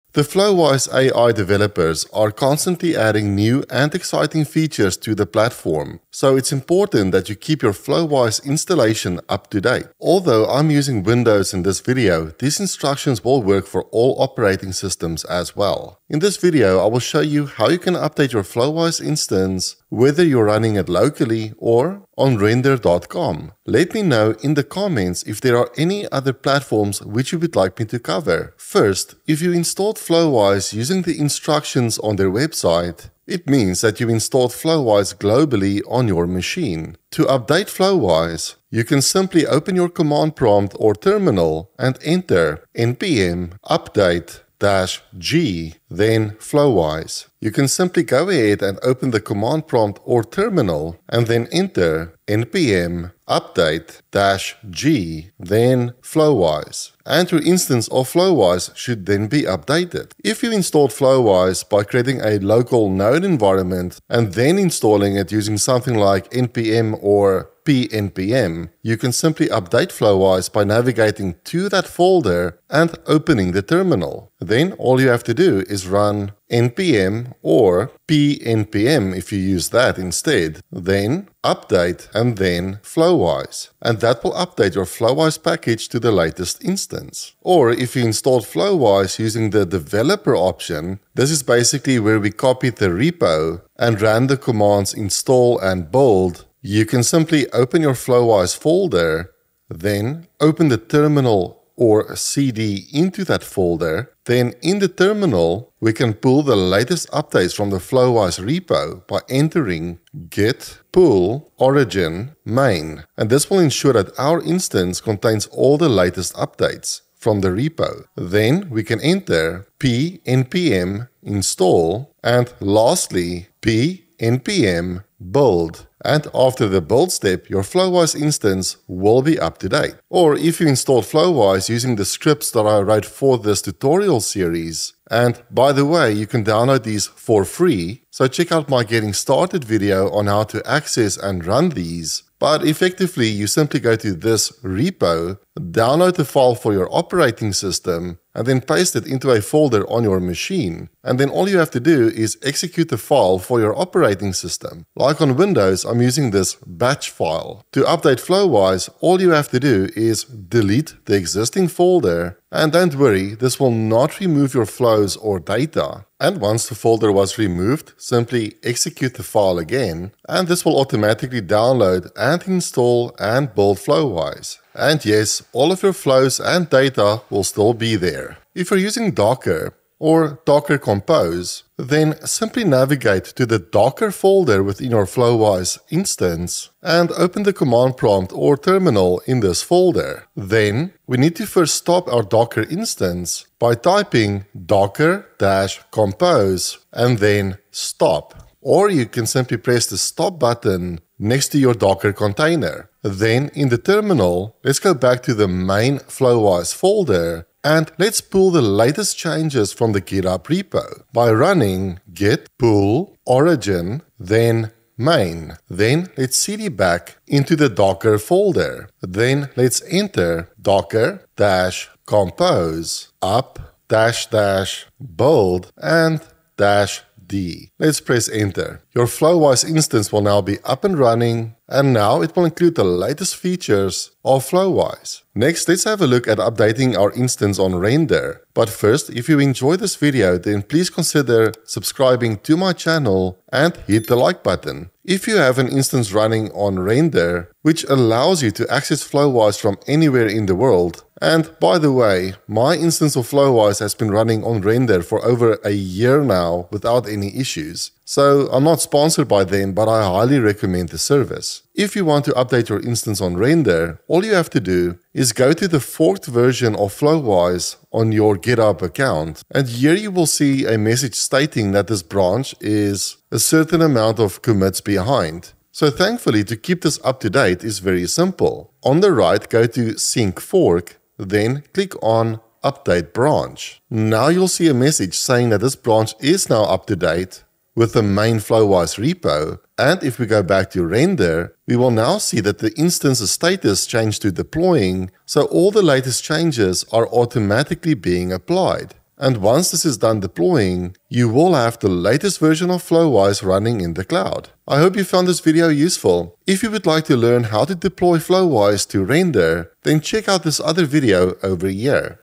How to Update Flowise AI (Local, Docker & Render)
In this video about updating Flowise we will go over the processes of updating Flowise in several situations.
These include updating Flowise AI globally, in a localized node environment, Docker and Render.com.

🕒 TIMESTAMPS:
00:00 - Intro
00:28 - "Global" Installation
01:00 - Localised Node Environment
01:27 - Developer Approach
02:09 - My Simple Script
02:58 - Docker Compose
03:55 - Updating Flowise on Render